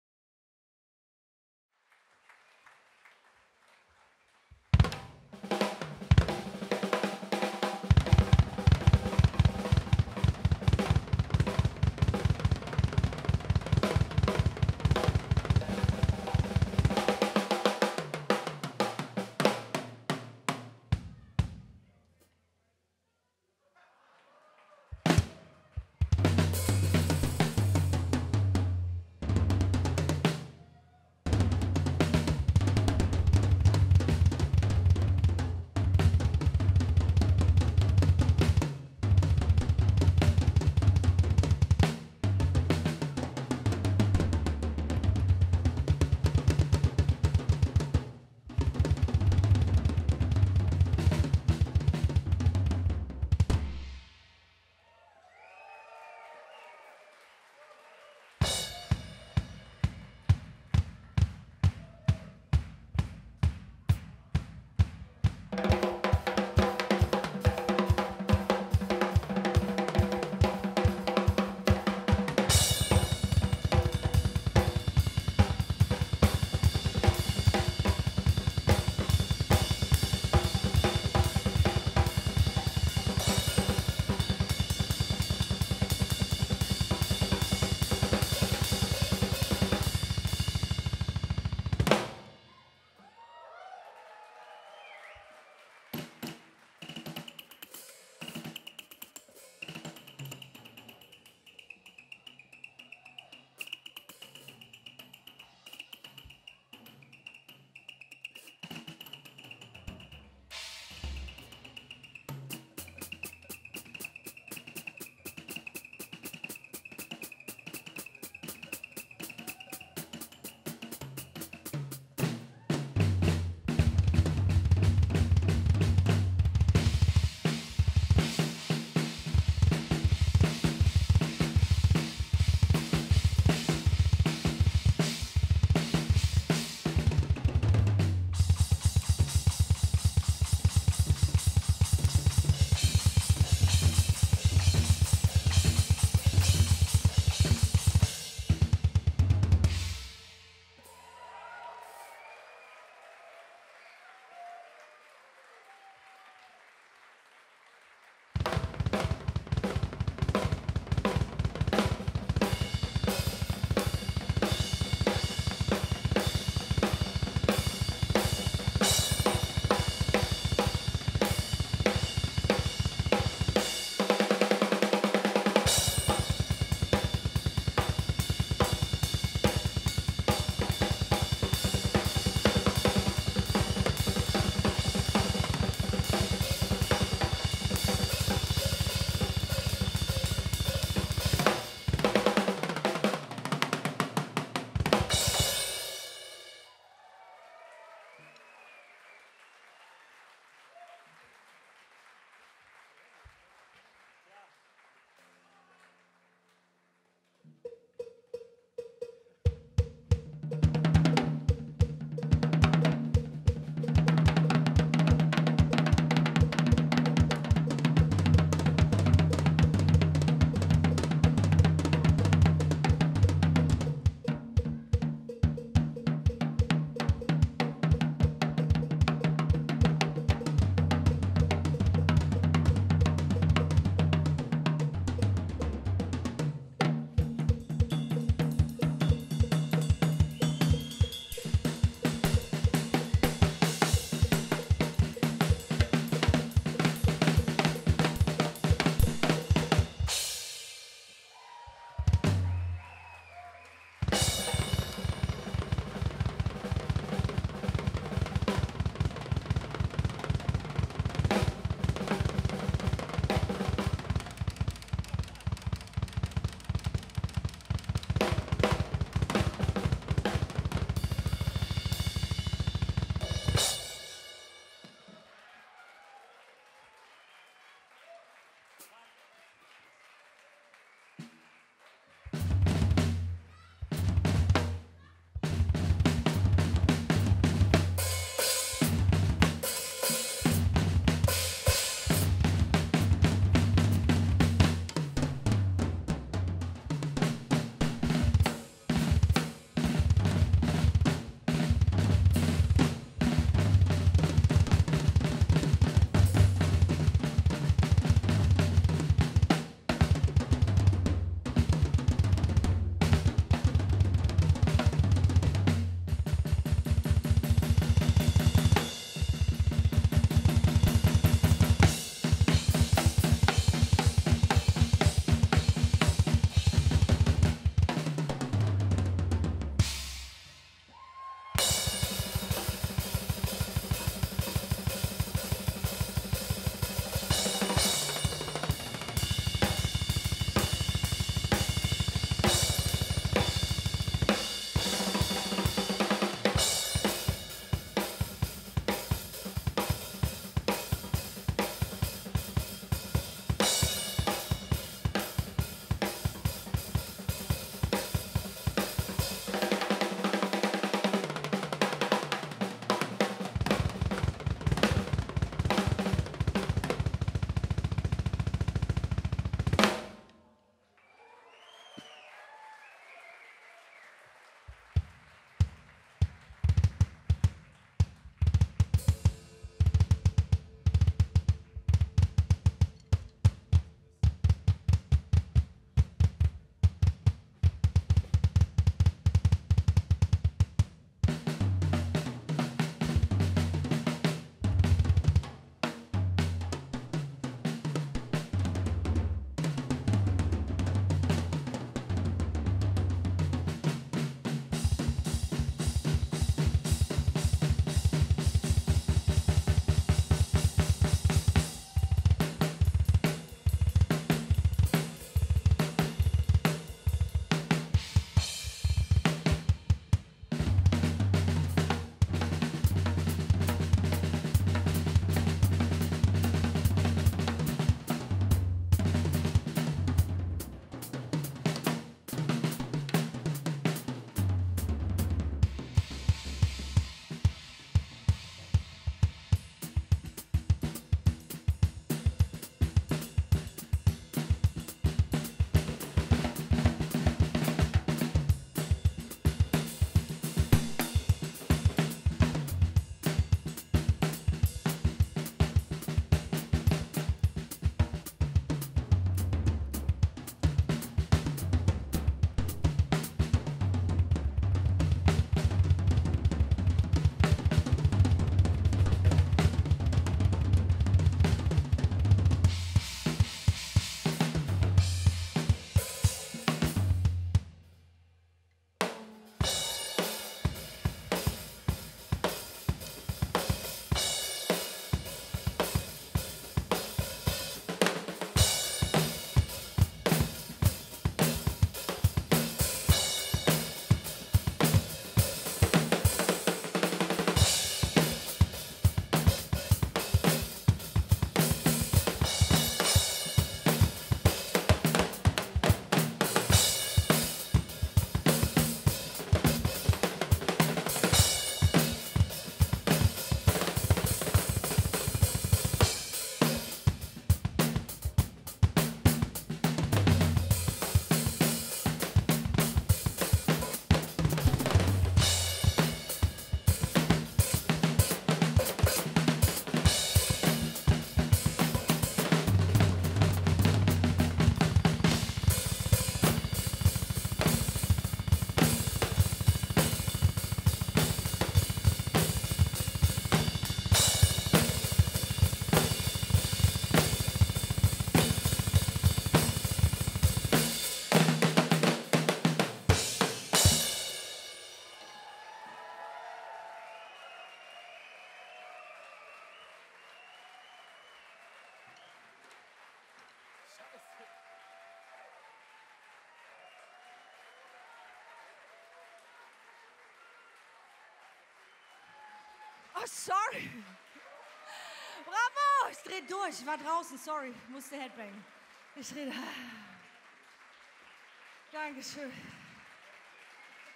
11Y drummer's focus Köln Drumbattle Alex Holzwarth & Hans-Martin Chemnitz
11Y Heartbeat - drummer's focus Köln feiert sein 11-jähriges Bestehen am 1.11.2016 in der Kantine Köln.
Ein absolutes Highlight der Show ist das Drumbattle von Hans-Martin Chemnitz, Schulleiter des drummer’s focus Köln, und seinem Freund und langjährigen Kollegen Alex Holzwarth, dem „Double Boss“ - so genannt wegen seiner abgefahren schnellen und exakten Double-Bass-Pedaltechnik.
Alex, Drummer von u.a. Rhapsody of Fire und Serious Black sowie Lehrer am drummer's focus München, und HMC liefern sich einen fulminanten Schlagabtausch, bei dem nicht nur die Stöcke fliegen - aber seht selbst...

Ton: Tobias Wendeler
Titelfoto: Monika Sandel

Sabian Cymbals
Beyerdynamic Mikrofonie
Vic Firth Sticks
Remo Drumheads

Pearl Midtown Black Cherry Glitter
Pearl Eliminator Redline Pedals
Zildjian Cymbals
Wincent Sticks

Weitere Videos folgen !
7.Dezember 2016